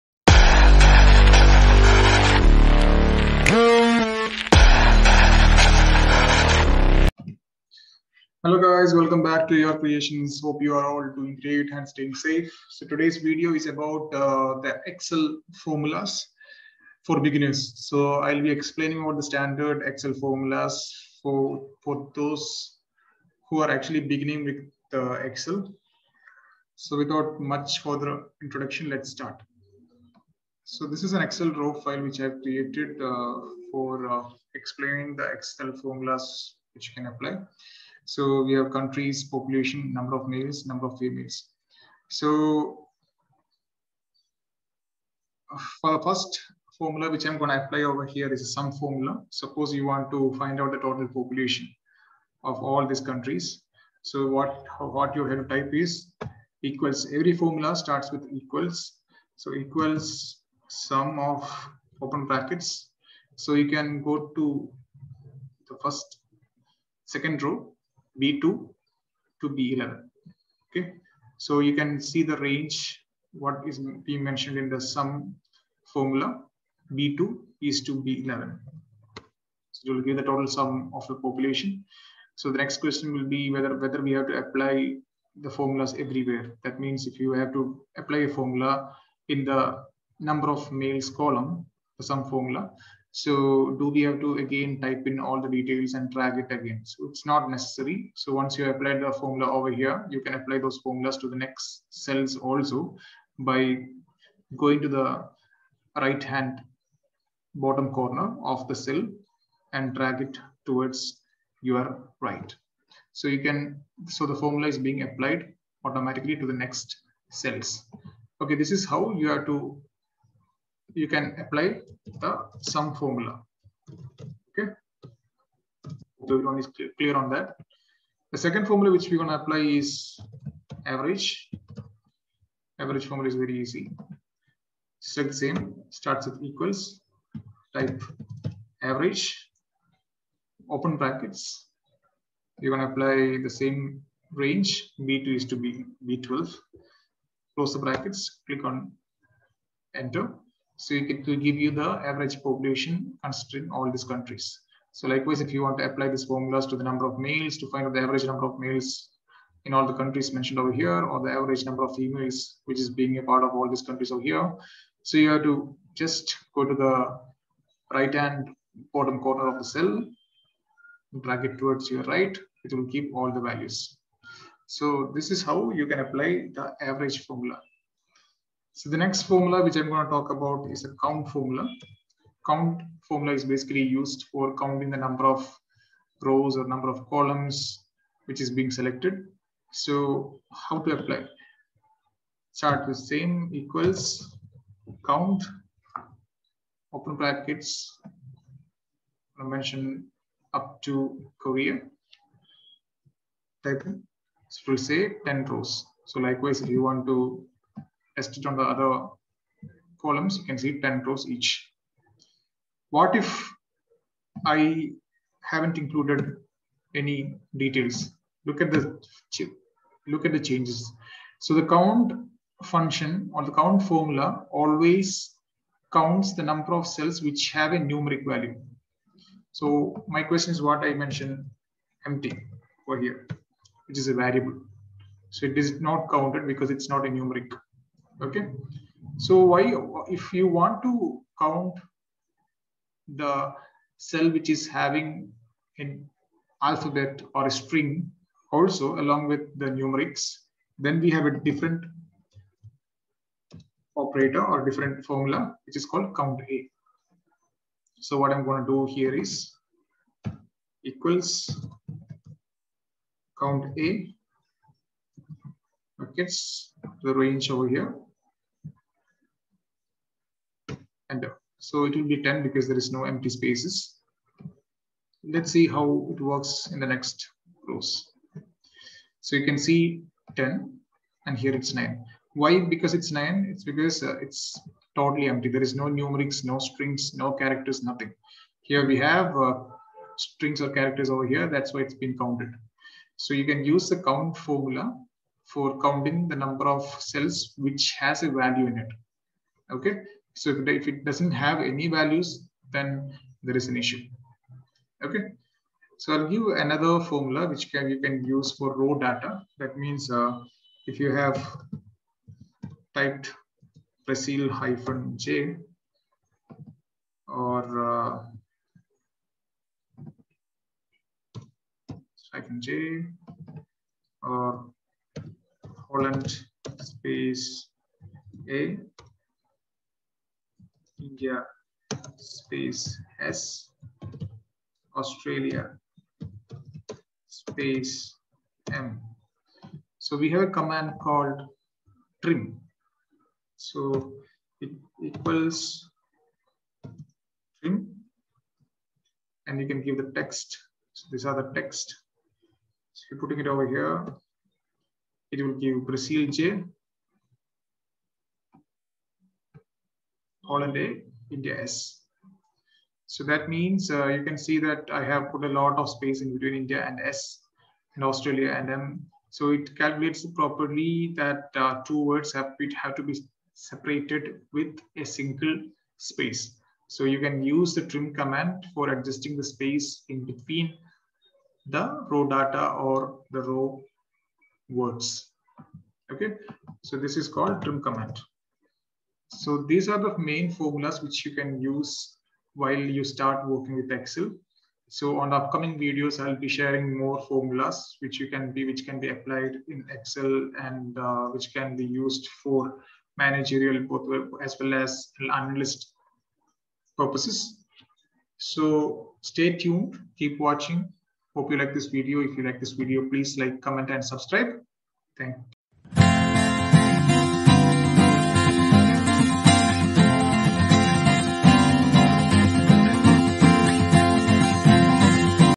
Basic Excel Formulas for Beginners | Excel Shortcuts | Microsoft Excel 2022
Basic Excel Formulas
1. Sum Formula
2. Count Formula
3. Drag and Use
4. Freeze Cell
5. Filter Formulas